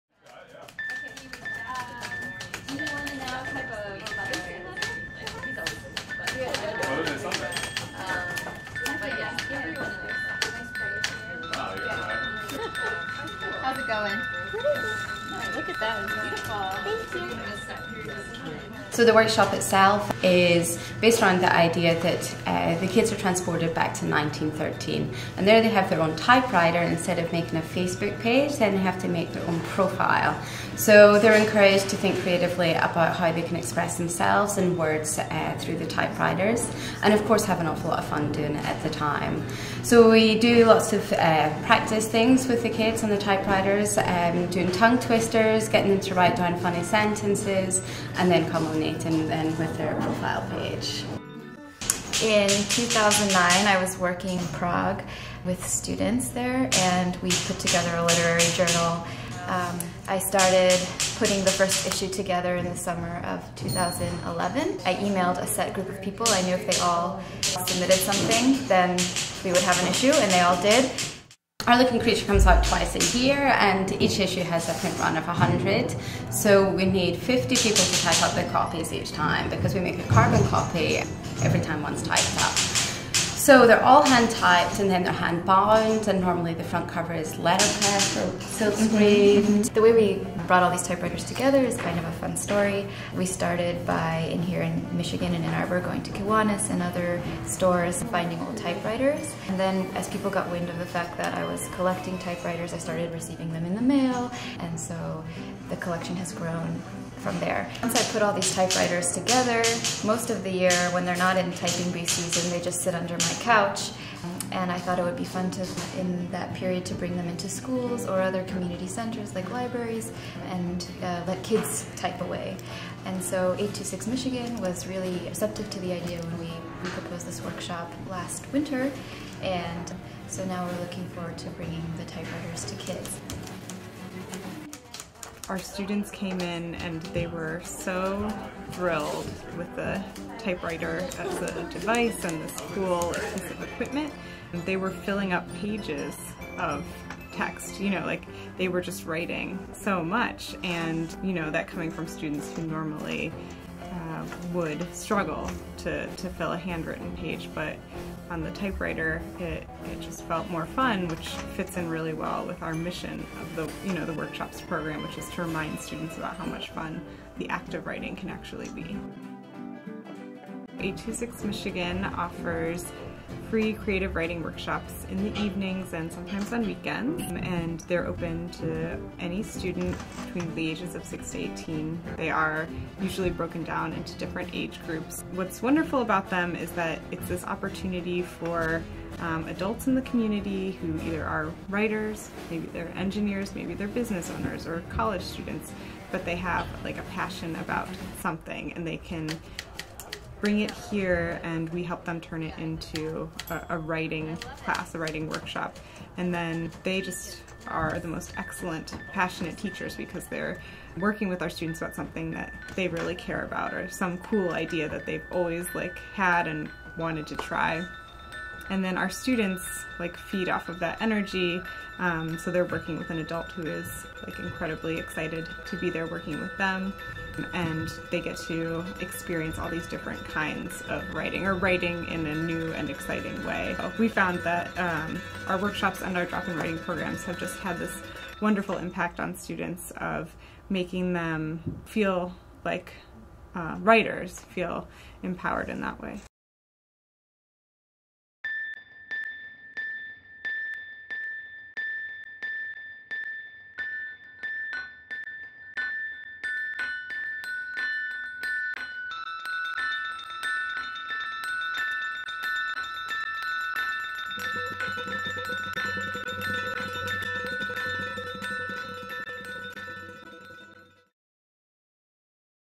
harlequin creature brings typewriters to kids
harlequin creature brings typewriters to kids and much creativity ensues!